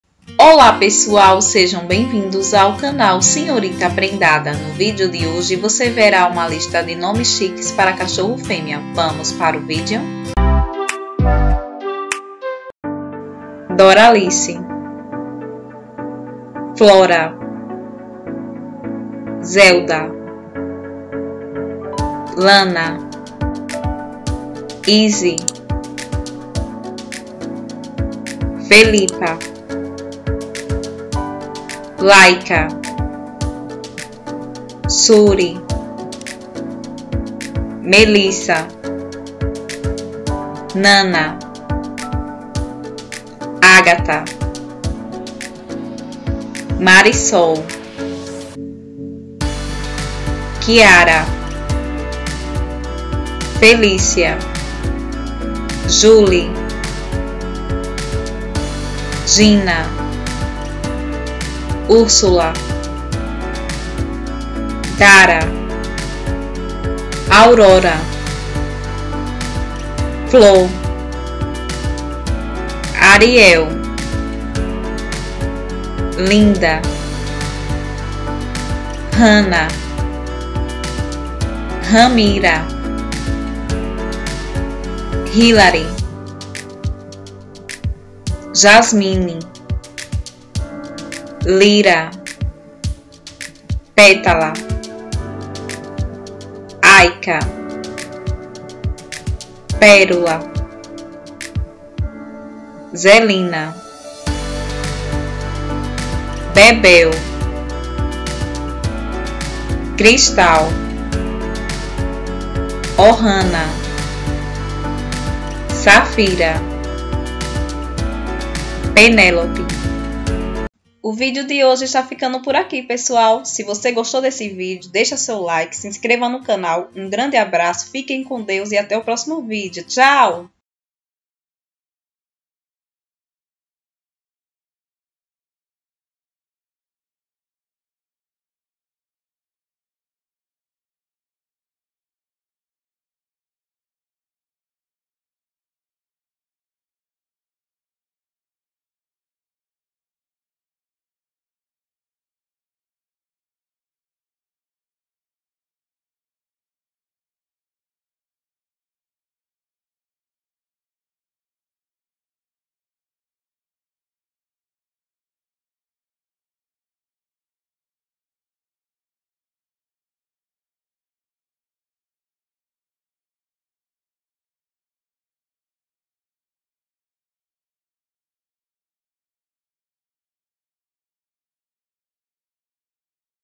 NOMES CHIQUES PARA CACHORRO FÊMEA-PARTE 02
Nesse vídeo você encontrará uma lista de opções de nomes de cachorro fêmea chique  que ajudará você a escolher o nome perfeito para a sua cadelinha.